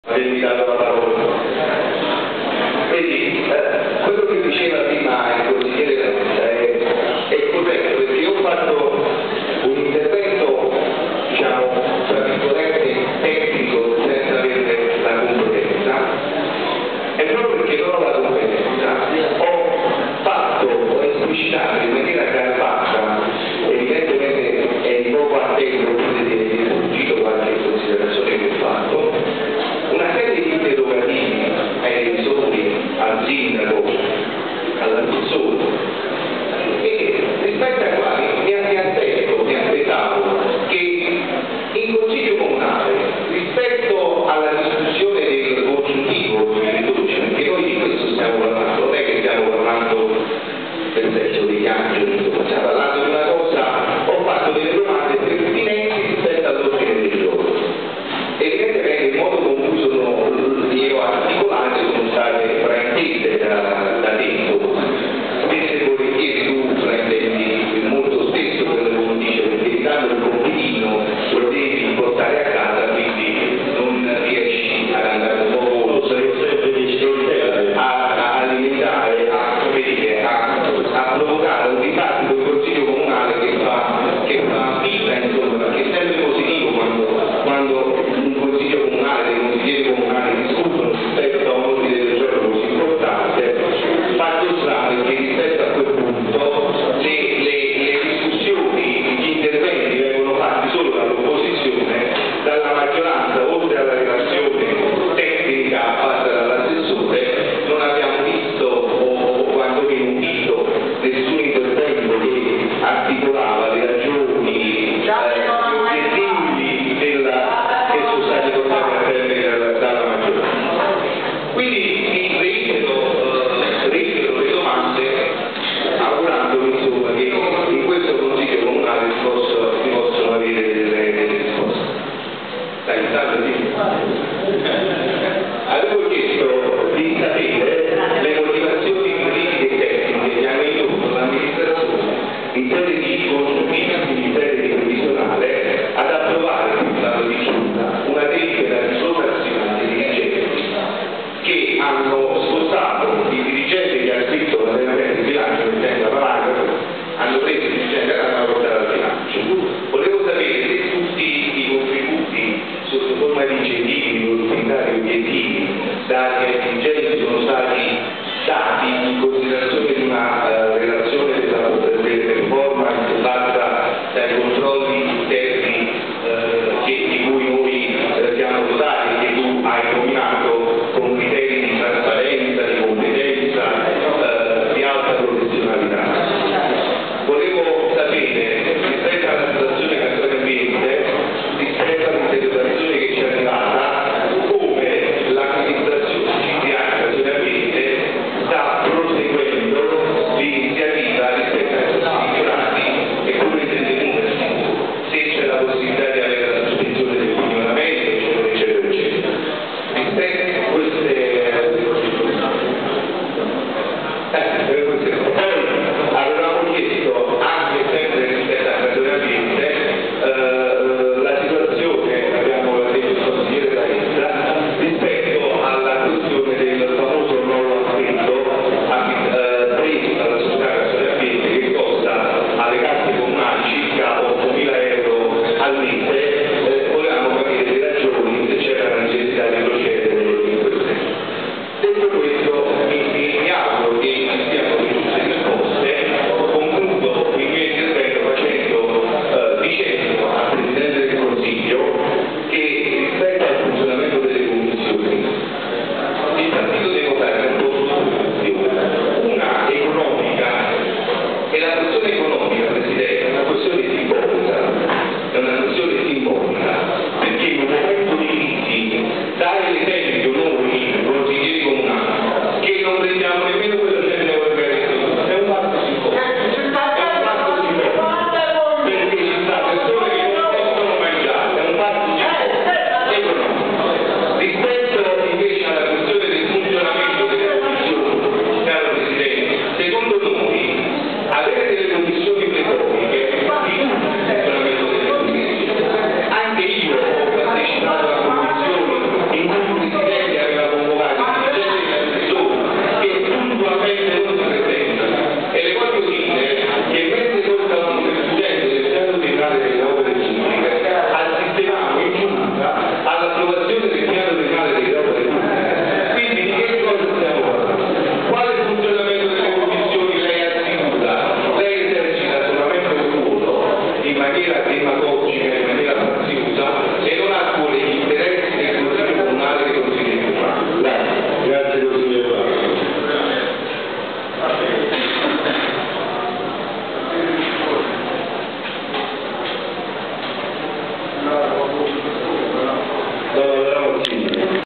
Casoria 08/05/2013 - Consiglio Comunale (11)
"Casoria Ieri & Oggi online" presenta:
Casoria 8 maggio 2013 il Consiglio Comunale è convocato in sessione ordinaria di prima convocazione alle ore 15,30, presso la sala Consiliare della Sede Comunale sita in P.za Cirillo, per la discussione e approvazione del seguente argomento :
Rendiconto della gestione anno 2012 -- Approvazione.
Intervento del consigliere Pino Balsamo (Pd)